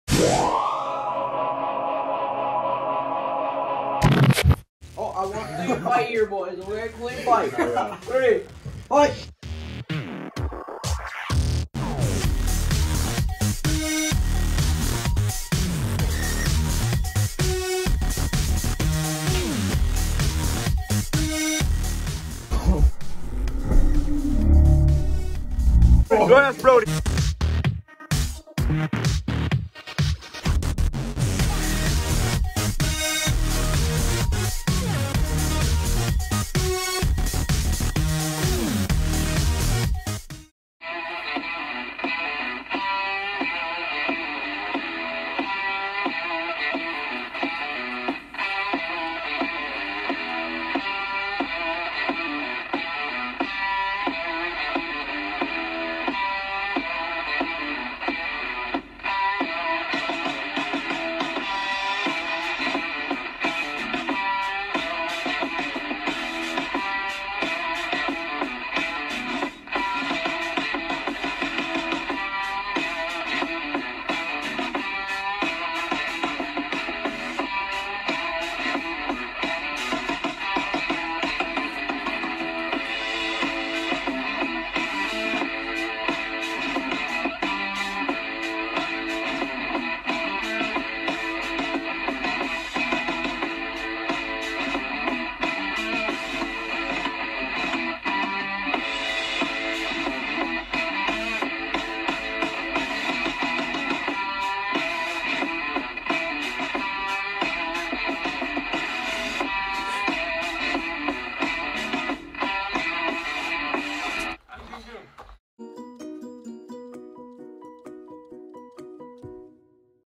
FIGHT NIGHT! Backyard Boxing
Watch Me and the Boys beat each other!